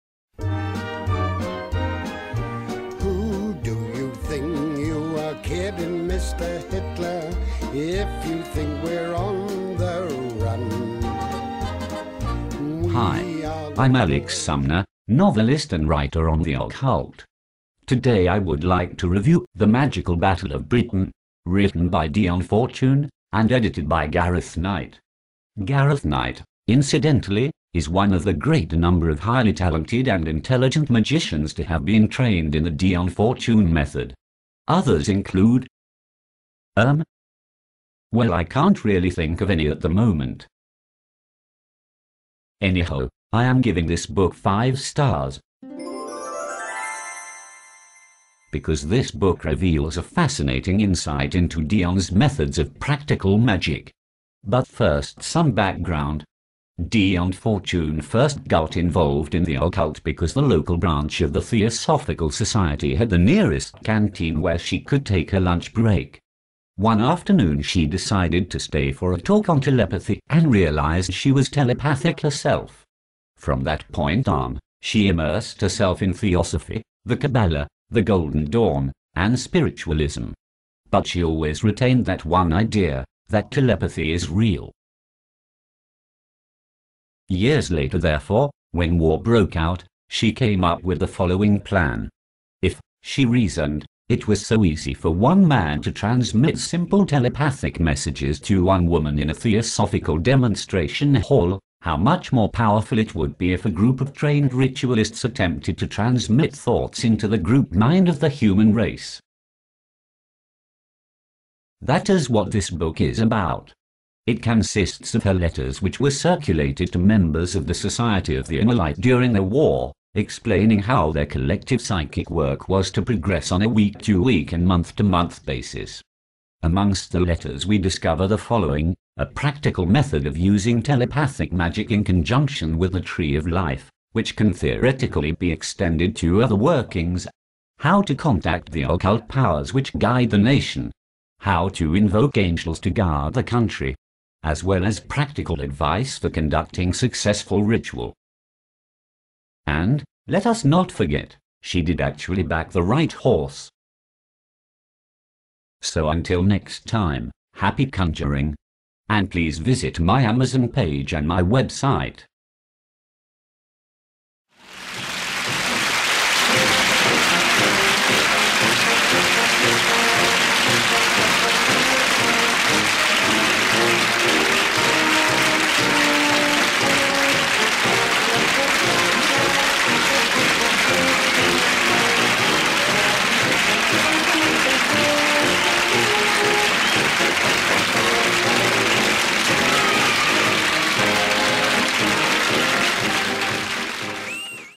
"The Magical Battle of Britain" by Dion Fortune, a review by Alex Sumner.
Novelist and writer on the occult Alex Sumner presents a video review of "The Magical Battle of Britain" by Dion Fortune (edited by Gareth Knight). Alex gives five stars to the book, as it is a fascinating account of Dion's practical magick.

"The Magical Battle of Britain" on Amazon

Alex Sumner's own books